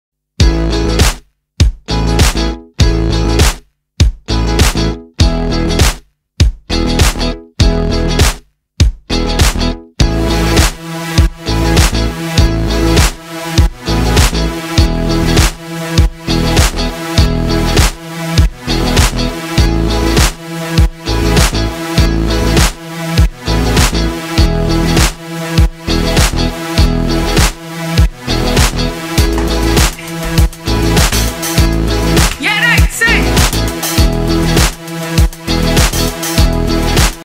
Toilet Roll Challenge While In Isolation
Members of the Barmy Army took on the toilet roll challenge.